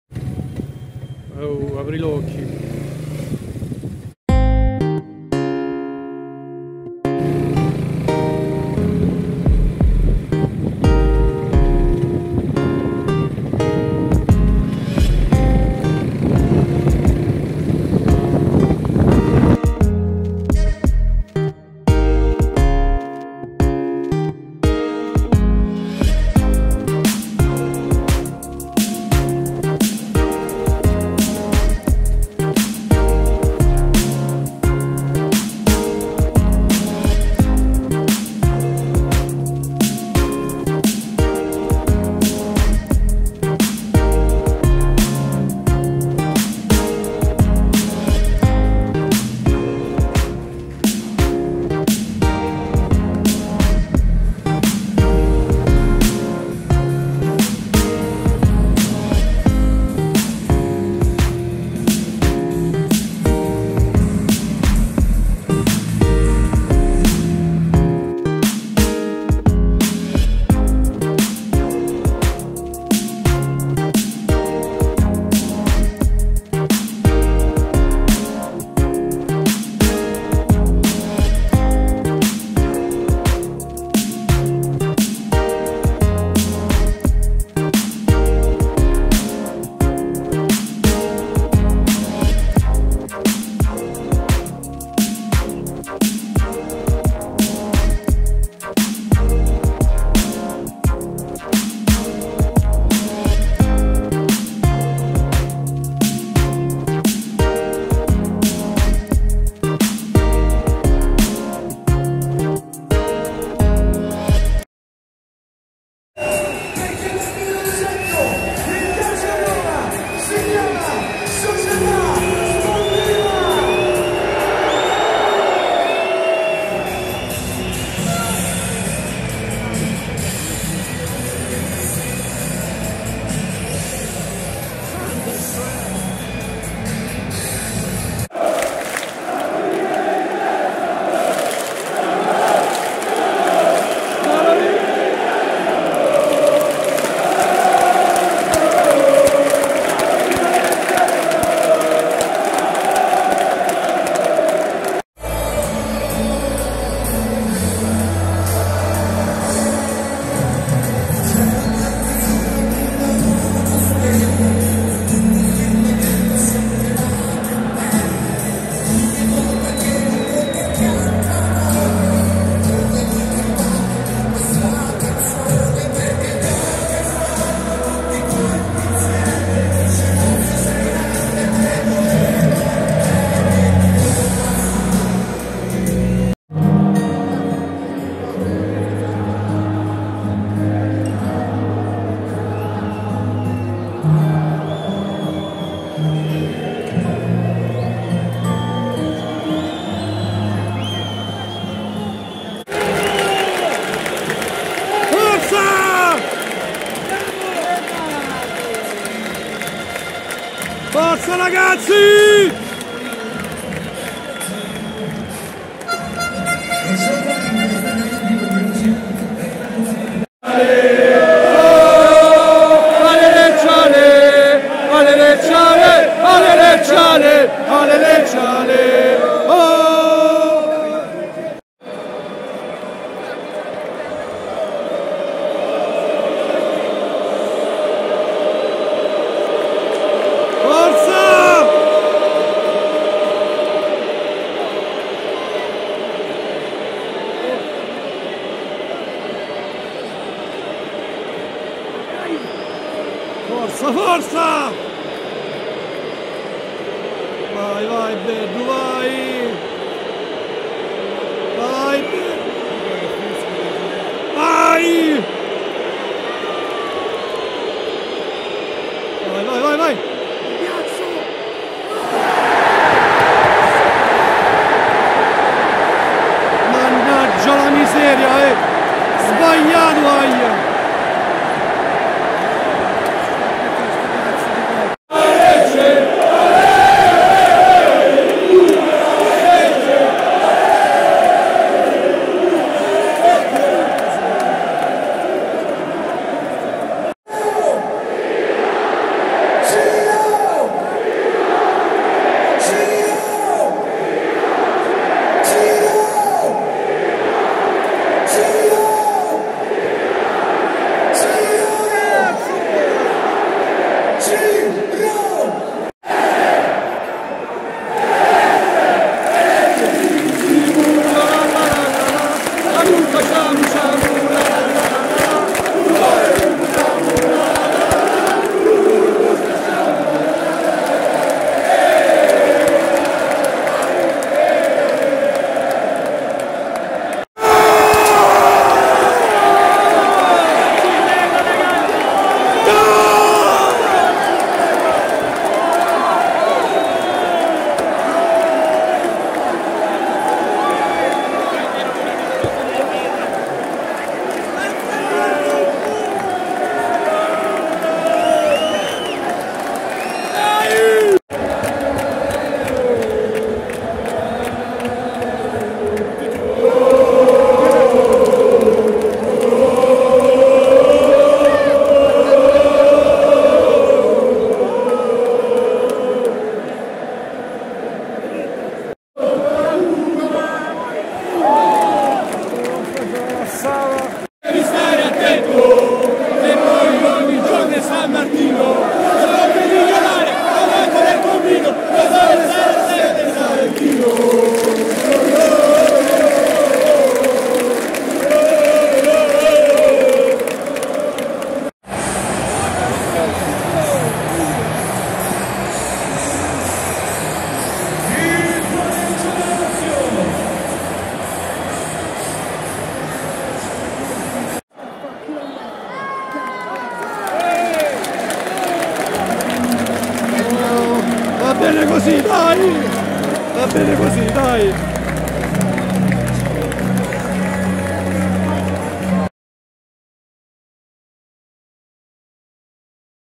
LAZIO - LECCE: TRASFERTA DEL 12/05/2023
Roma, 12/05/2023 - Stadio Olimpico. LAZIO - LECCE 2 - 2